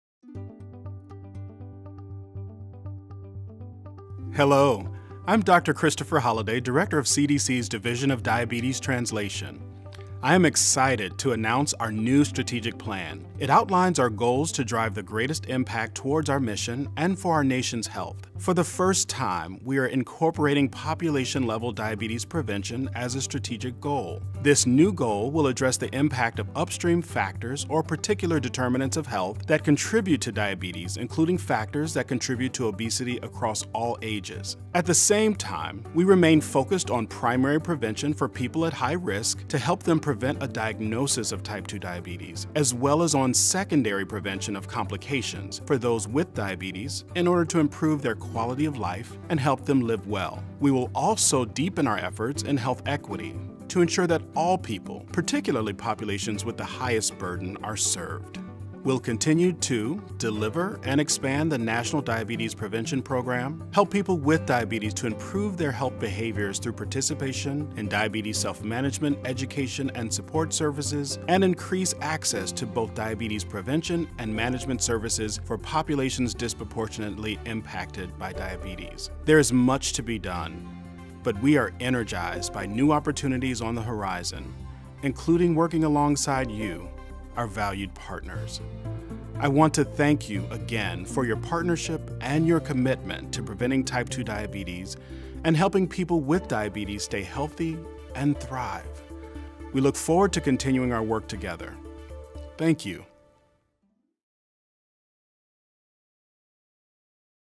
CDC’s Division of Diabetes Translation 2022-2027 Strategic Plan Video
Dr. Christopher Holliday discusses the Division of Diabetes Translation's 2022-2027 Strategic Plan and how the division is working to reduce the burden of diabetes in the United States.